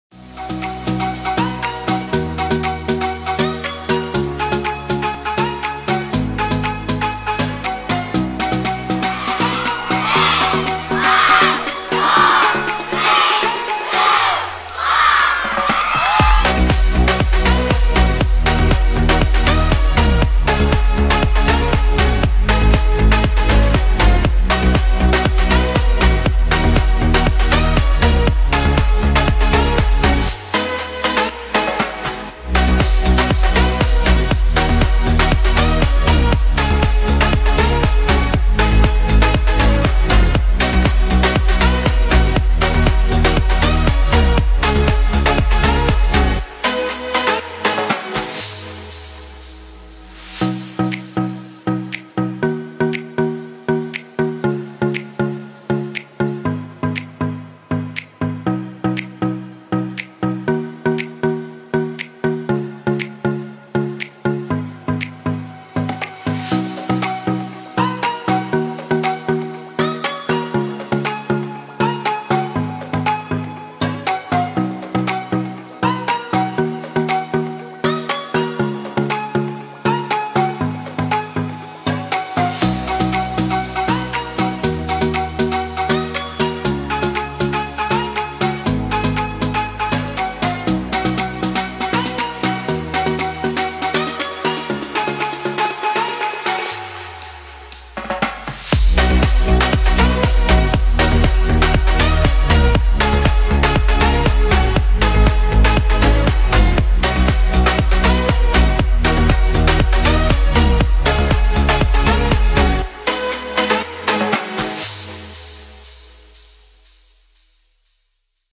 Shipwrecked VBS 2018
At 'Shipwrecked VBS' we had over 170 kids in attendance and more than 70 volunteers! On the final day, 45 children chose to give their lives to Christ!

For more information about our children's programs go